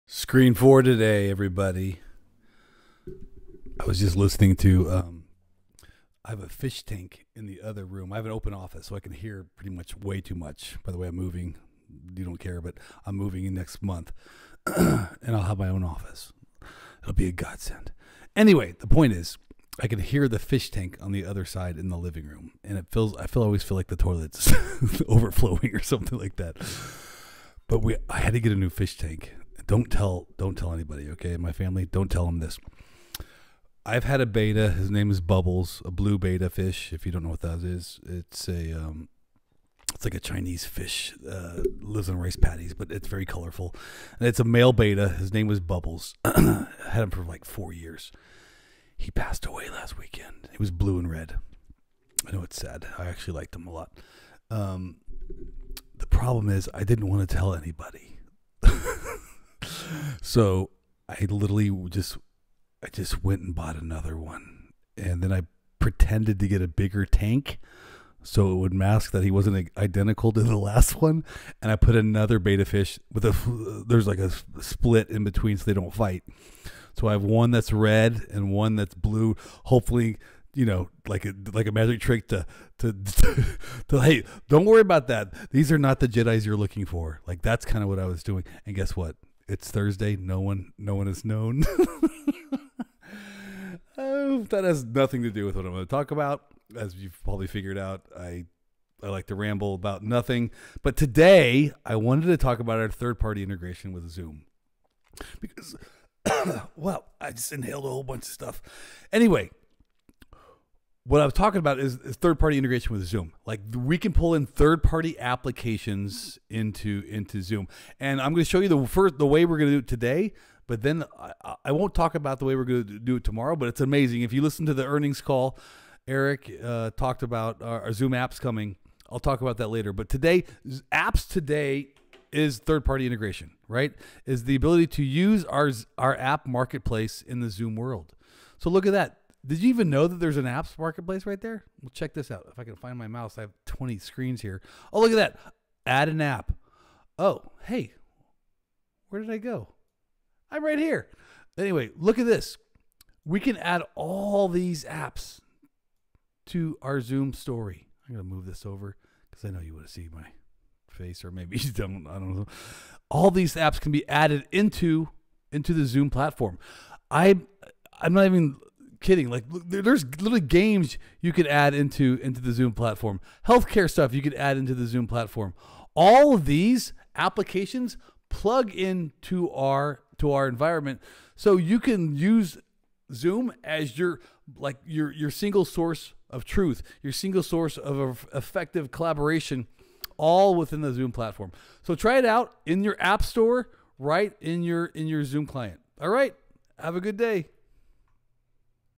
Zoom App Marketplace
Watch an example of how to pull in 3rd party apps into your Zoom Meetings!